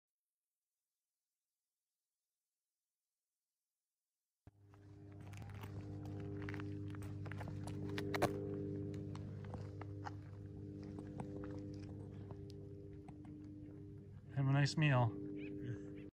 Tarantula Hawk and it’s prey
Watch as a Pepsis Wasp AKA Tarantula Hawk drags a tarantula away after paralyzing it. It will be taken to its brood nest and used as living food ; a single egg is laid on the prey, hatching to a larva which eats the still-living prey. The larva will feed voraciously, avoiding vital organs for as long as possible to keep the spider alive. After several weeks, the larva pupates. Finally, the wasp becomes an adult and emerges from the spider's abdomen to continue the life cycle. The Tarantula Hawks powerful sting is considered one of the most painful insect stings in the world.